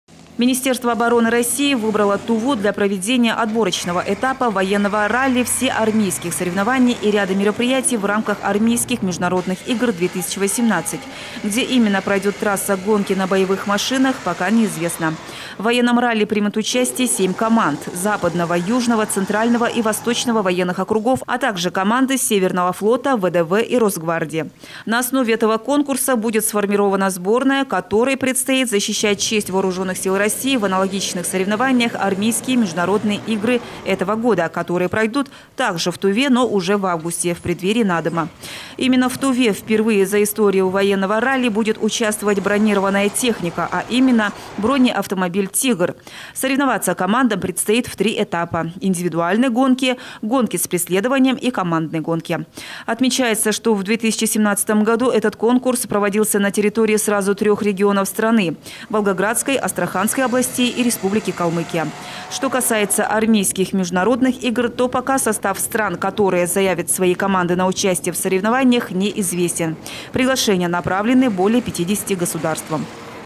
ВОЕННОЕ РАЛЛИ В ТУВЕ
Министерство бороны России выбрало Туву для проведения отборочного этапа «Военного ралли» Всеармейских соревнований и ряда мероприятий в рамках Армейских международных игр-2018. Где именно пройдет трасса гонки на боевых машинах пока неизвестно. В «Военном ралли» примут участие семь команд: Западного, Южного, Центрального и Восточного военных округов, а также команды Северного флота, ВДВ и Росгвардии. На основе этого конкурса будет сформирована сборная, которая предстоит защищать честь Вооруженных Сил России в аналогичных соревнованиях Армейские международные игры-2018, которые пройдут также в Туве, но уже в августе, в преддверии Наадыма.